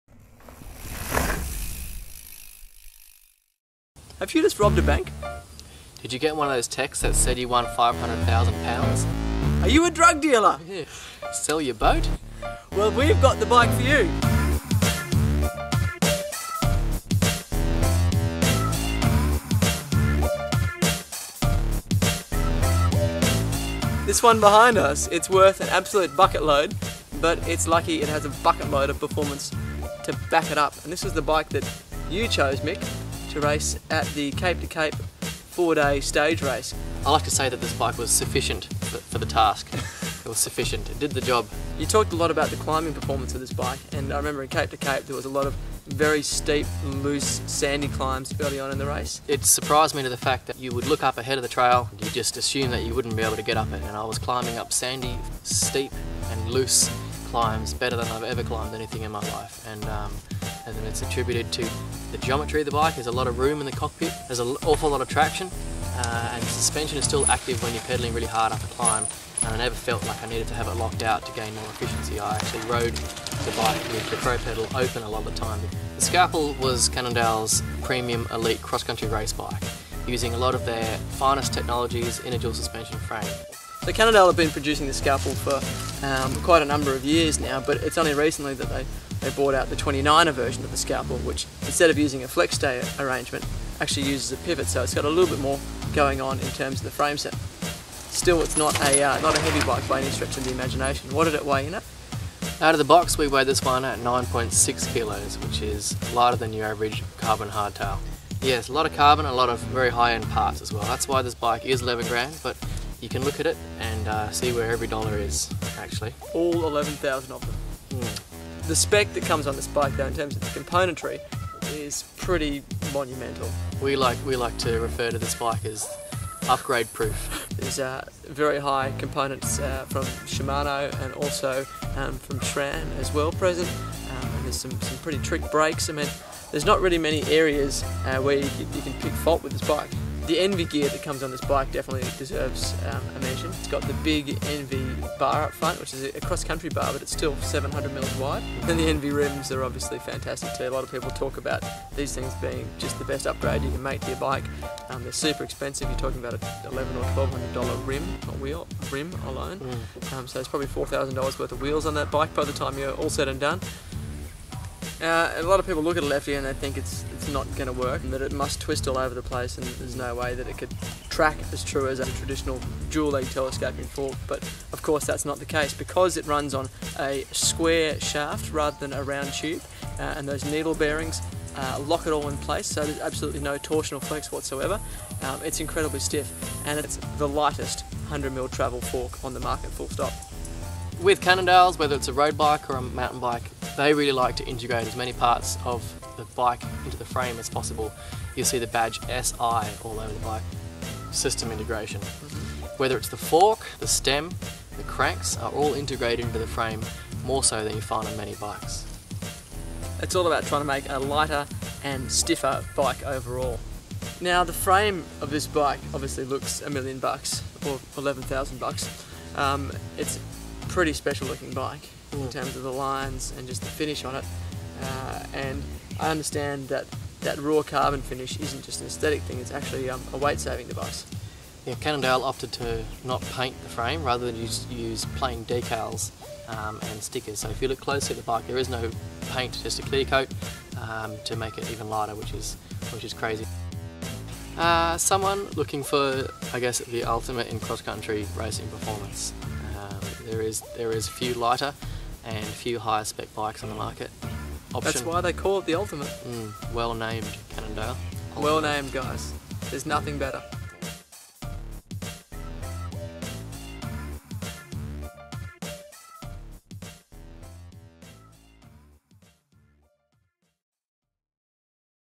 Bike Test: Cannondale Scalpel Ultimate 29 Review - Flow Mountain Bike
Flow Mountain Bike reviews the Cannondale Scalpel Ultimate 29 - one of the most expensive mountain bikes on the planet.